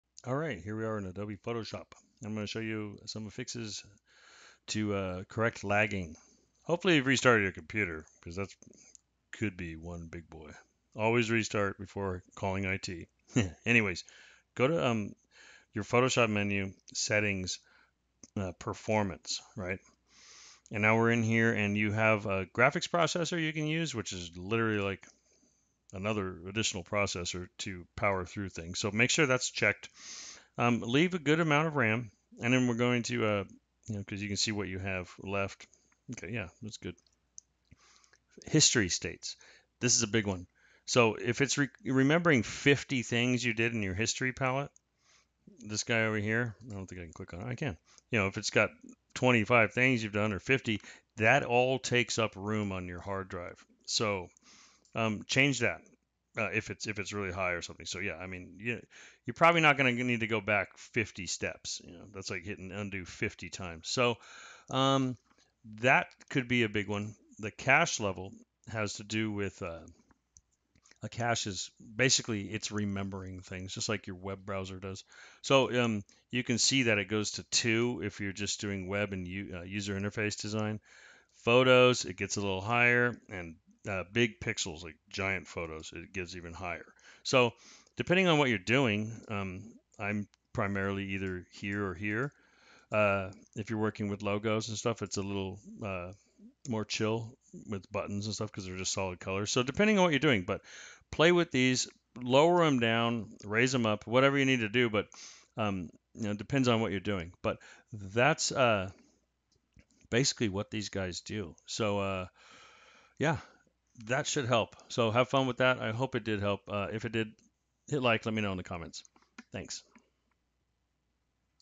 Photoshop Lagging or Slow? 5 Fixes in Under 2 Minutes
If Photoshop feels slow, laggy, or your tools are delayed — this quick tutorial walks you through 5 ways to instantly boost performance.

📌 What you'll learn:

How to toggle Graphics Processor acceleration

How to manage RAM allocation

Reduce History States for lighter memory load

Use Cache Level presets to match your workflow

What Cache does and when to optimize it manually

These tips are especially useful if you're editing large PSDs, working with lots of layers, or running Photoshop on an older machine. Speed it back up in just a few clicks.

Works on both Mac and Windows PCs